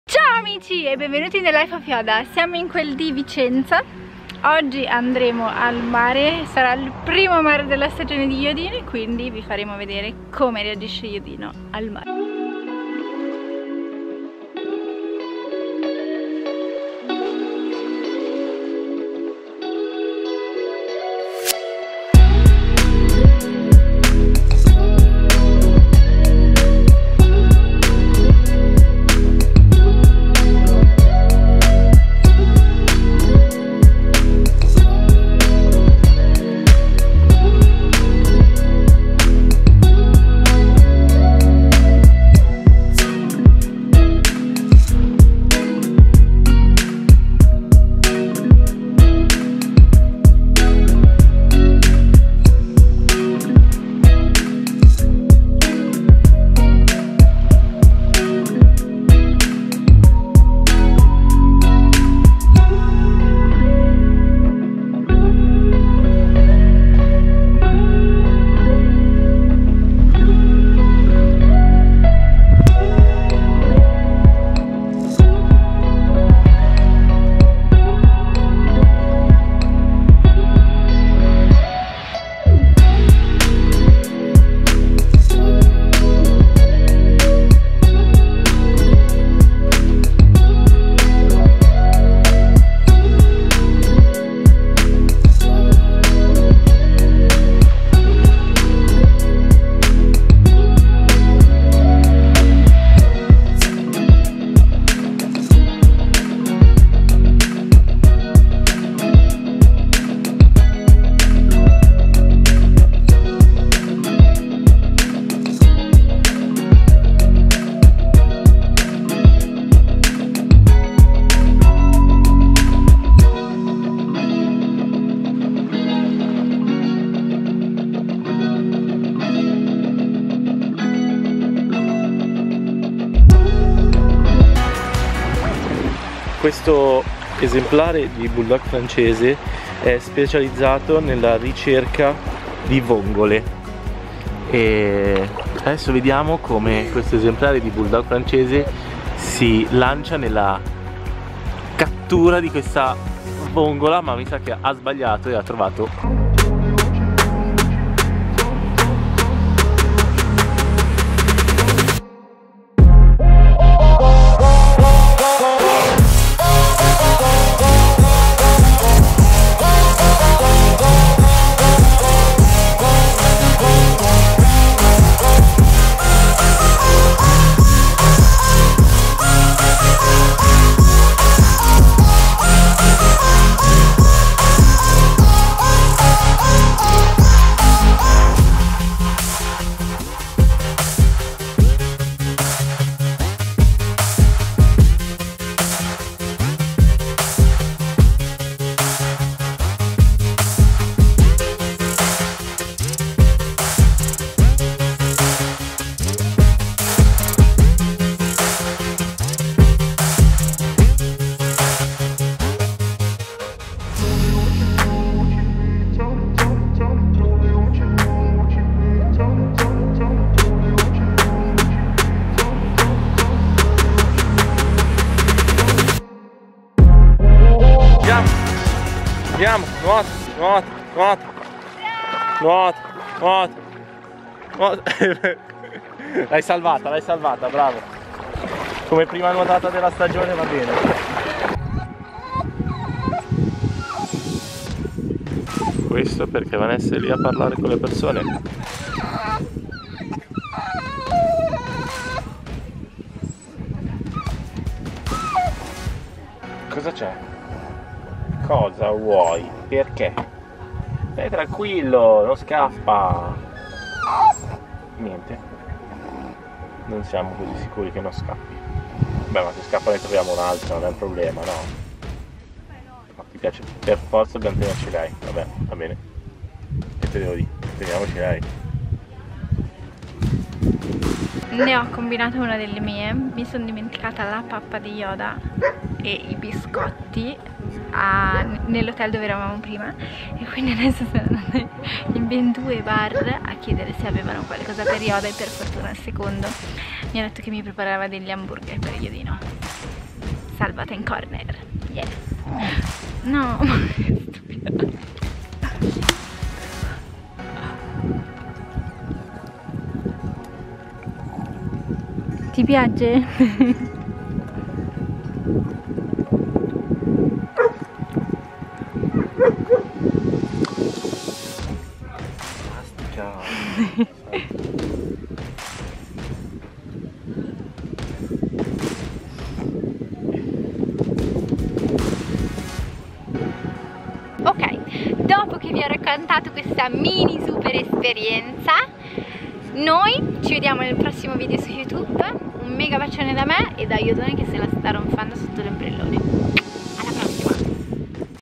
PORTO IL MIO CANE AL MARE🌊| primo bagno della stagione per Yoda
Ciao amici! Oggi vi terremo compagnia con questo mini video del primo bagno di Yoda della stagione! 😎🌊